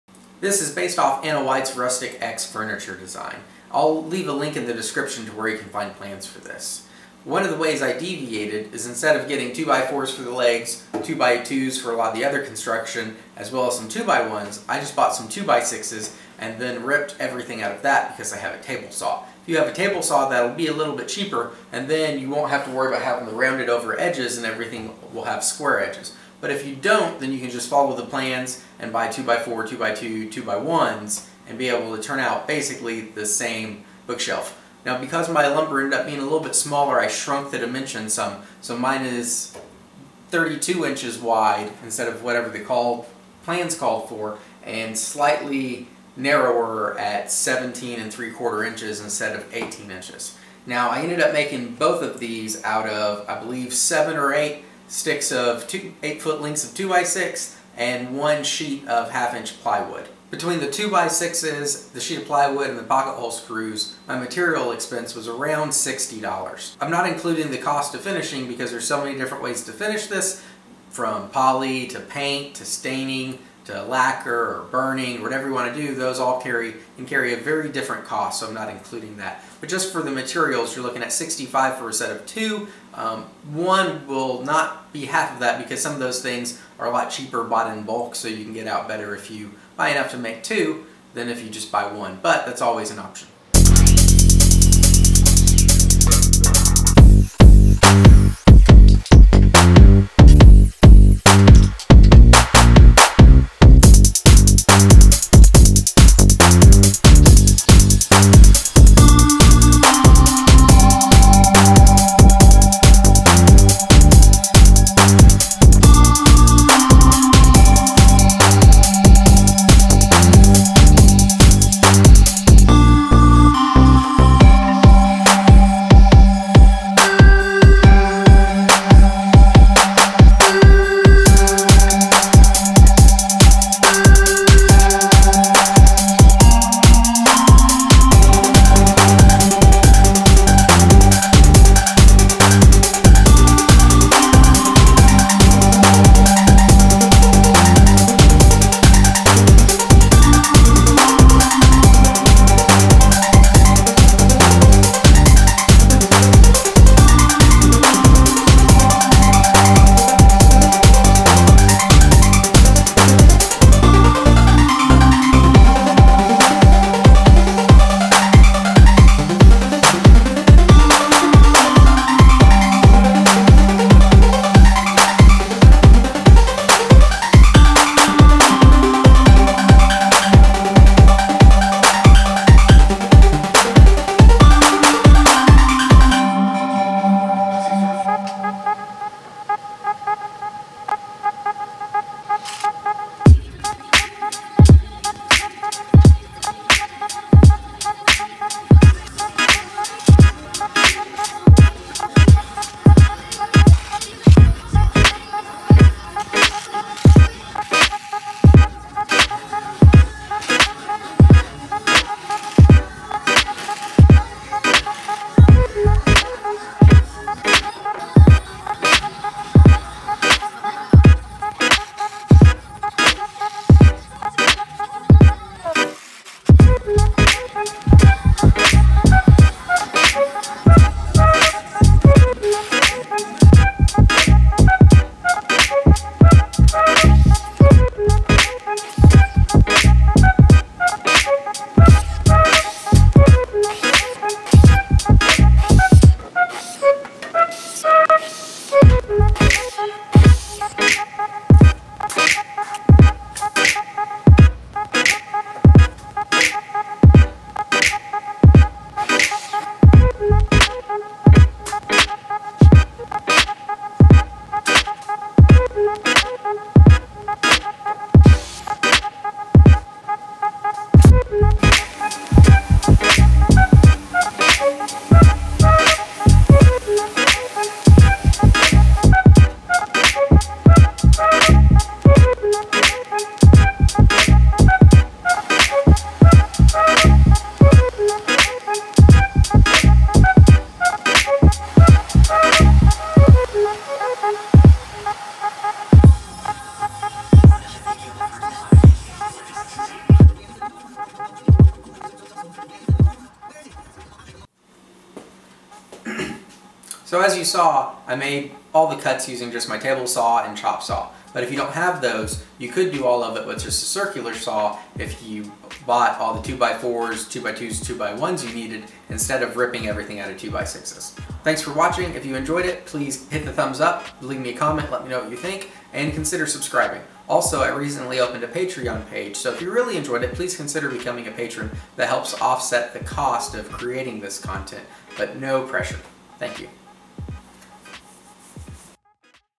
Ana White's DIY Rustic X Bookshelves // How to
Need a quick way to make stylish bookshelves? This is a good one. The best part is this can all be made from construction lumber from a big box store. And if you own a table saw, you can save some cash by ripping everything out of 2x6s.

Table Saw
Miter Saw
Drill / Driver: Makita 18v
Random Orbit Sander
MAP Torch

Blades:
Ripping Blade
Crosscut Blade
Plywood Blade
Bandsaw Blades